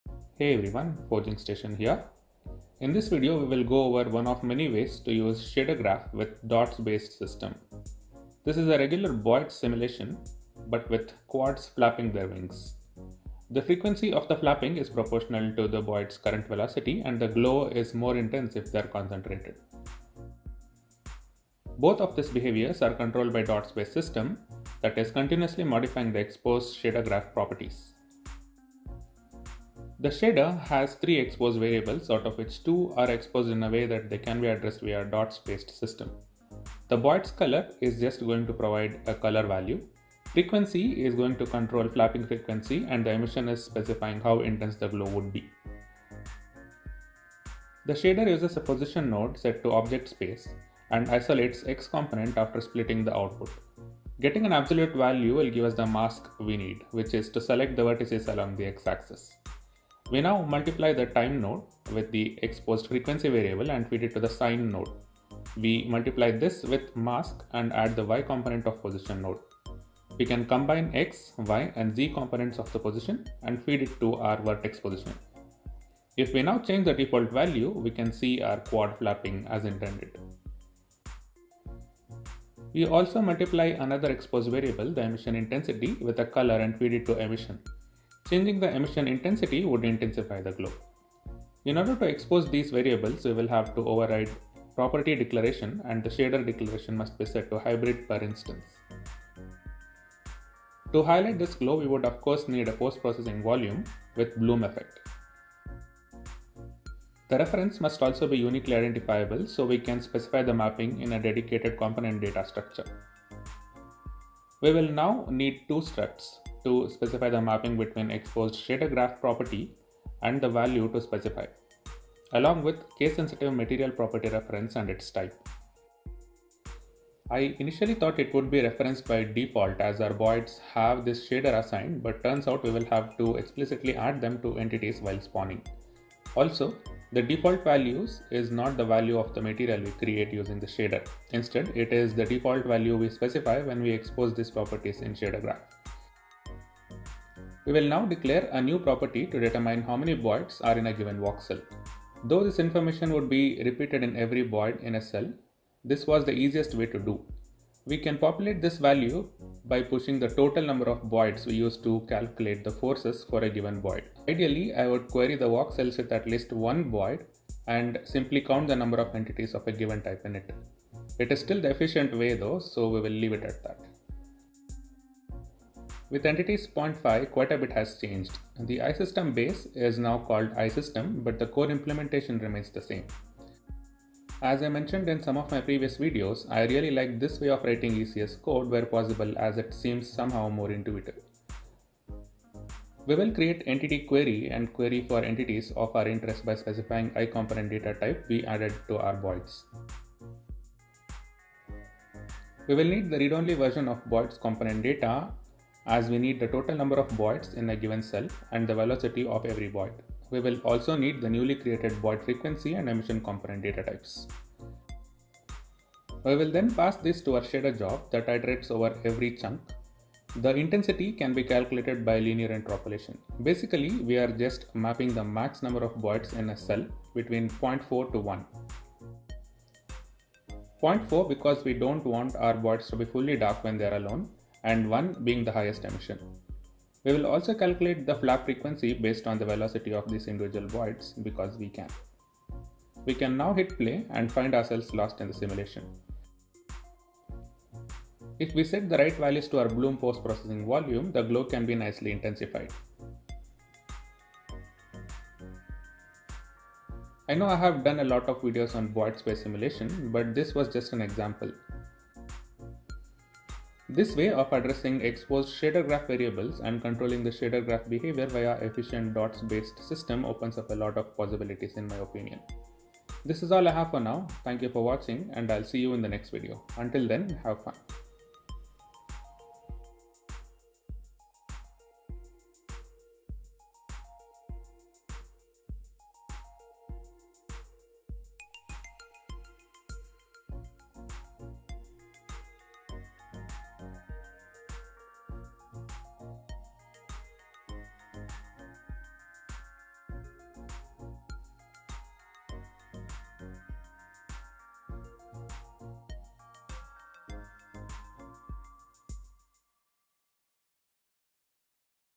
Shader Graph & Unity DOTS - Flocking Simulation
In this video, we will go over one of many ways to use shader graphs with DOTS based system. This is a regular boids simulation but with quads flapping their wings. The frequency of the flapping is proportional to the boids current velocity and the glow is more intense if they are concentrated.
Both of these behaviors are controlled by a DOTS-based system that is continuously modifying the exposed shader graph properties.